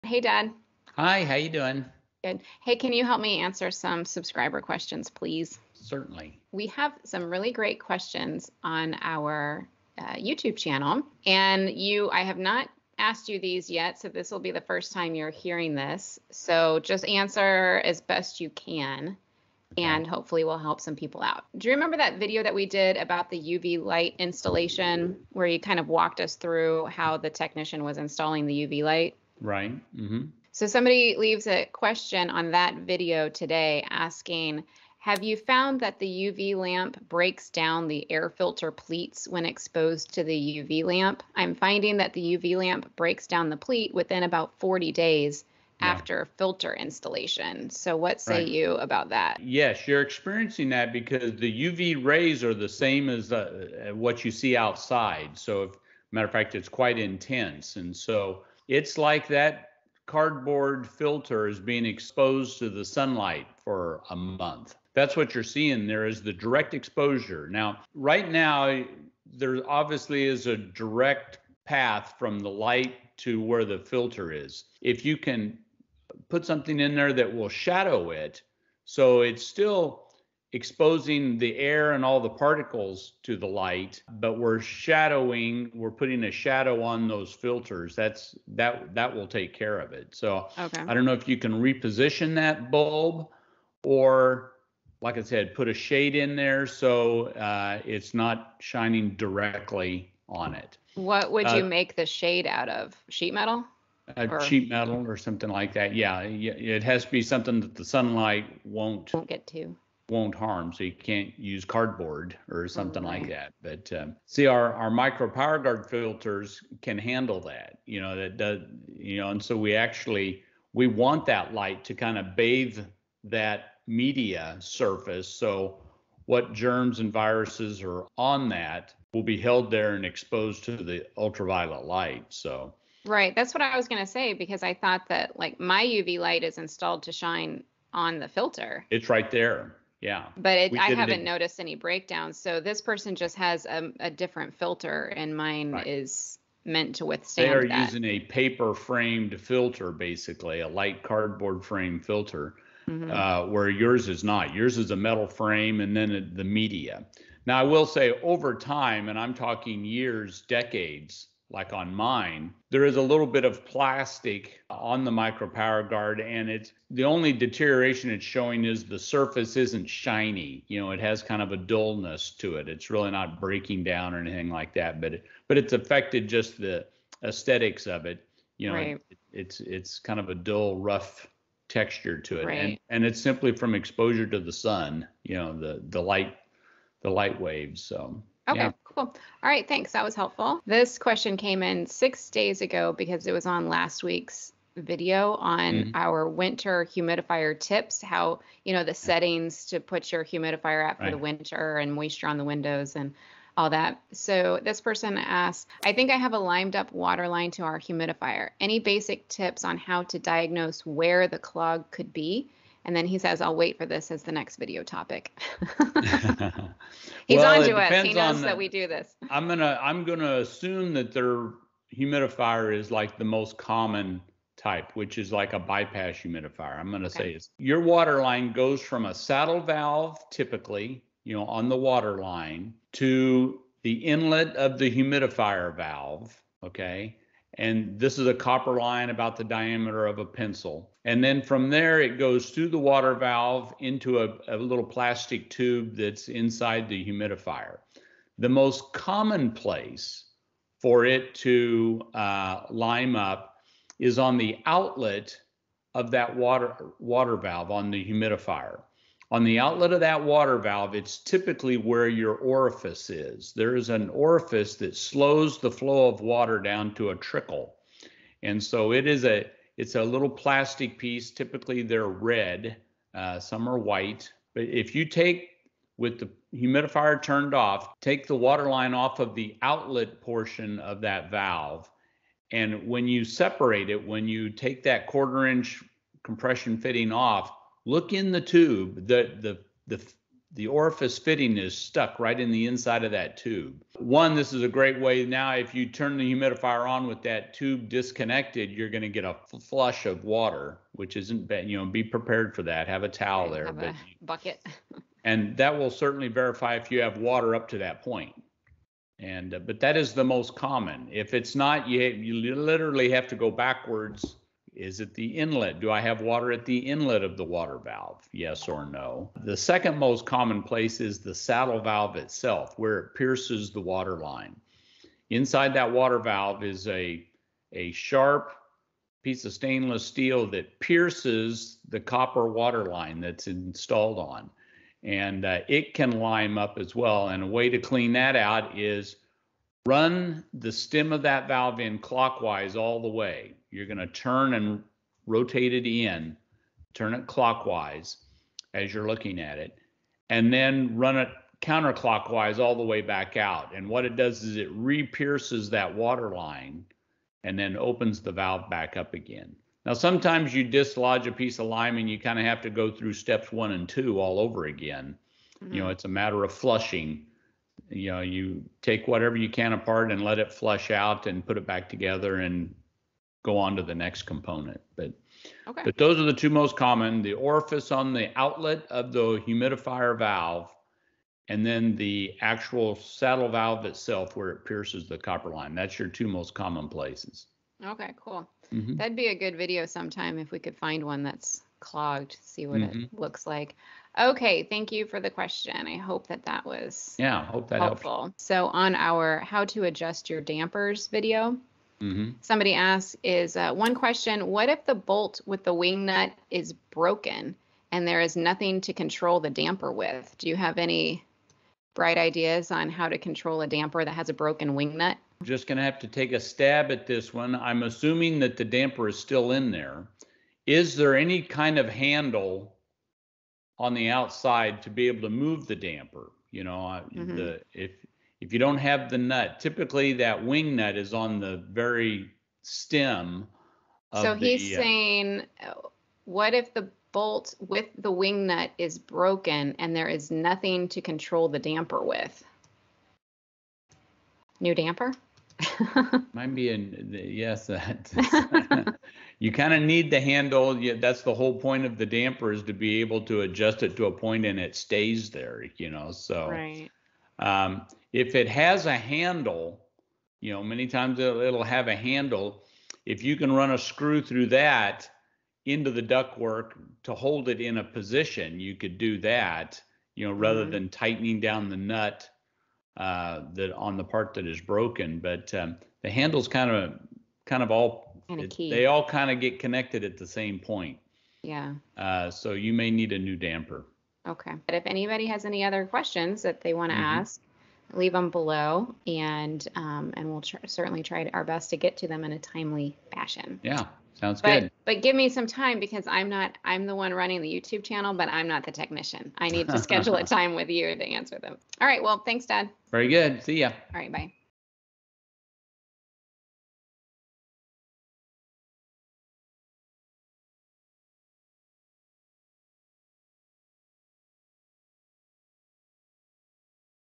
YOUR HVAC QUESTIONS ANSWERED BY AN HVAC EXPERT | UV Lights, Humidifier Clogs and Broken Dampers
Today we are answering some subscriber questions we have been asked recently.  Here are the questions we answer in the video:

HVAC Question 1: "Have you found that the UV lamp breaks down the air filter pleats when exposed to the UV lamp? I am finding that the UV lamp breaks down the pleat within about 40 days after filter installation (20X20X4)."

HVAC Question 2: "I think I have a limed up water line to our humidifier. Any basic tips on how to diagnose where the clog could be?"

HVAC Question 3: "Hi thank you for the great video. One question what if the the bolt with wing nut is broken and there is nothing to control the damper with?"

If you have any other questions about your residential HVAC system, please leave them in the comments.

Our Recommendations:
CO Detector
Handy furnace filter cover